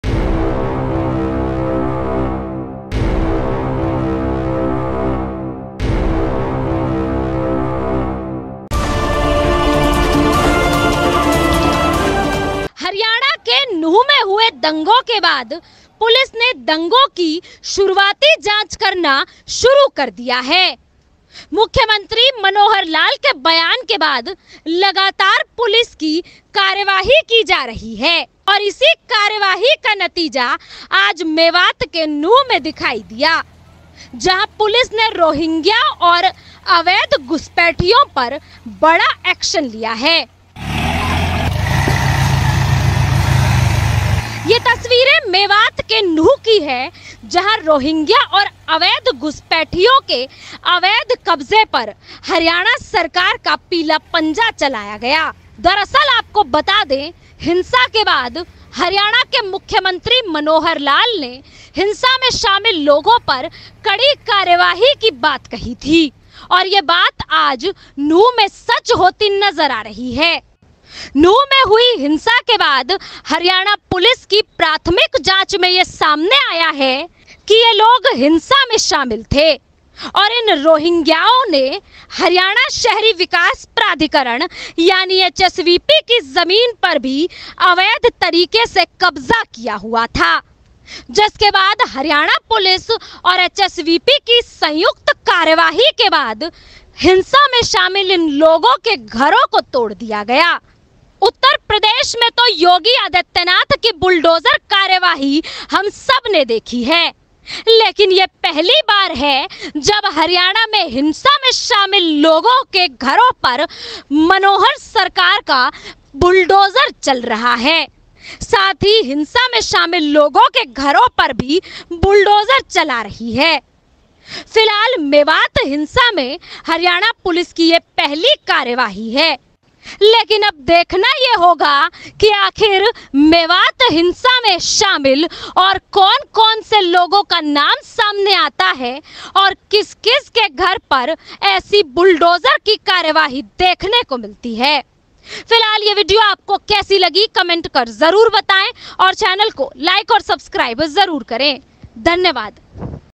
मेवात हिंसा में बुलडोजर की एंट्री, क्या खट्टर में किया अपना वादा पूरा?
हरियाणा के न्यू में हुए दंगों के बाद पुलिस ने दंगों की शुरुआती जांच करना शुरू कर दिया है मुख्यमंत्री मनोहर लाल के बयान के बाद लगातार पुलिस की कार्यवाही की जा रही है और इसे कार्यवाही का नतीजा आज मेवात किन्नू में दिखाई दिया जहां पुलिस ने रोहिंग्या और अवैध घुसपैठियों पर बड़ा एक्शन लिया है